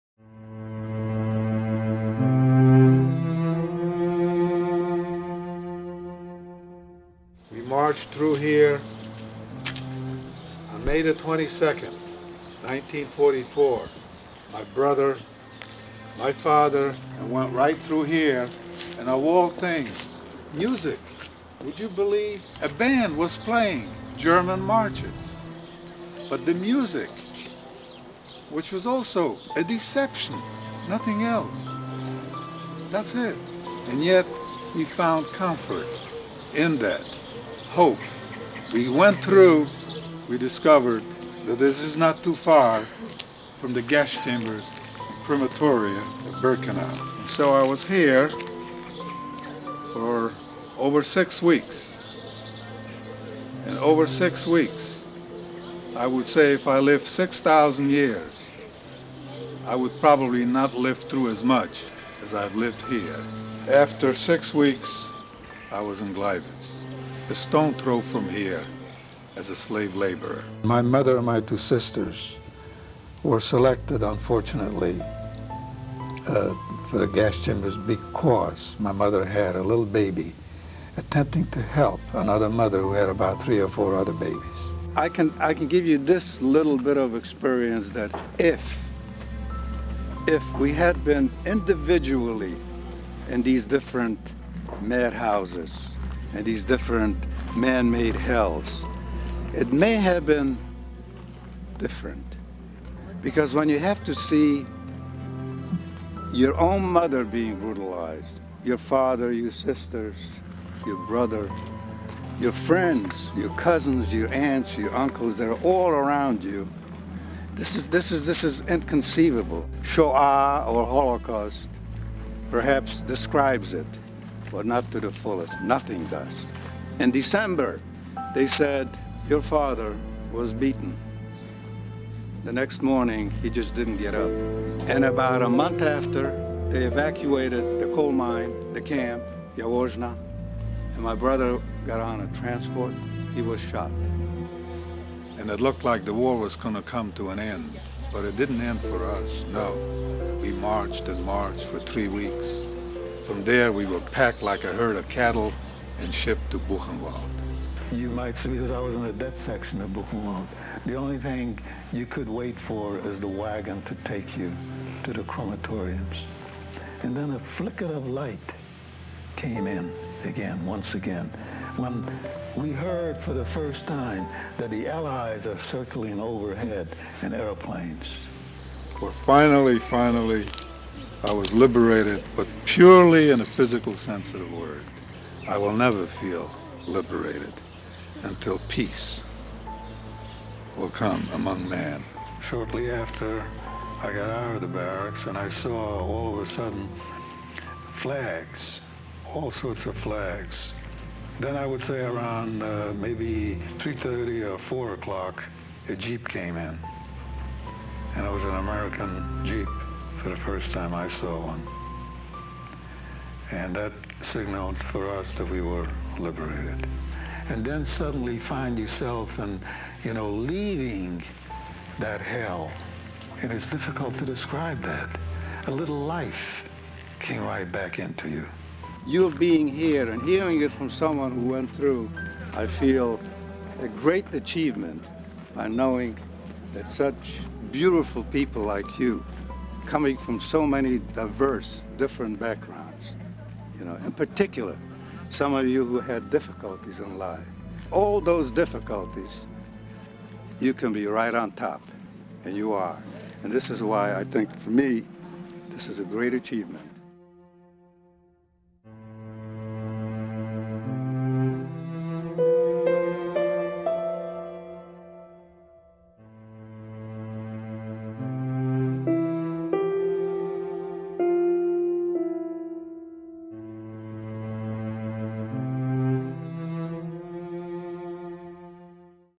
Liberation 75th Anniversary - Live to Tell
On April 11, 1945, at 2:30 p.m., misery, torture, starvation and death were to come to an end in Buchenwald.  That was the moment to remember ! Liberation... In Honor of Mel Mermelstein - survivor of Auschwitz-Birkenau, liberated from Buchenwald.